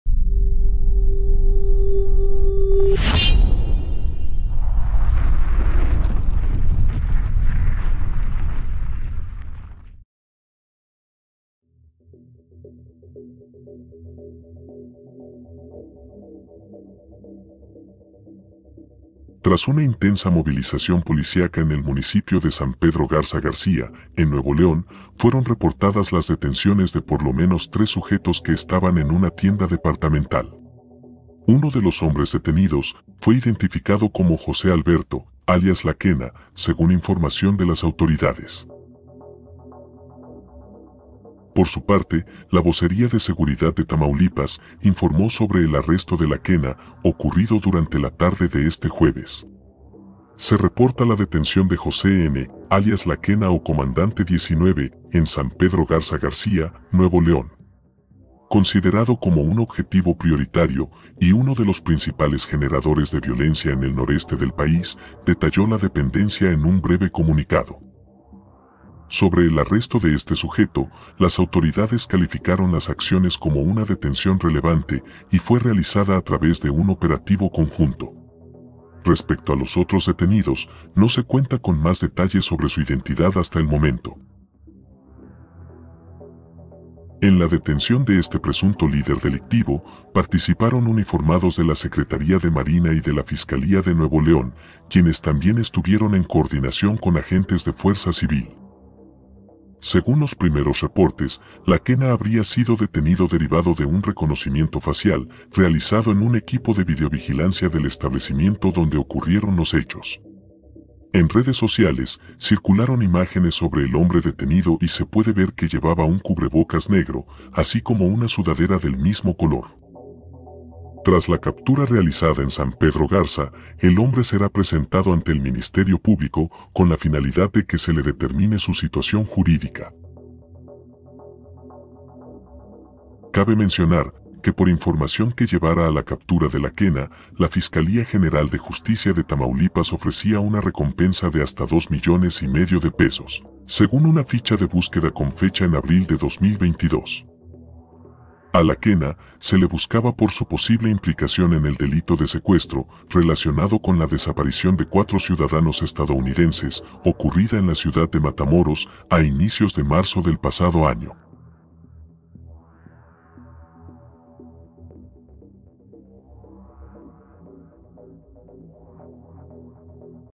La detención de "La Kena" en Nuevo León
En una intensa movilización policiaca, autoridades confirmaron que fueron detenidos tres sujetos en el municipio de San Pedro Garza García, en el estado de Nuevo León.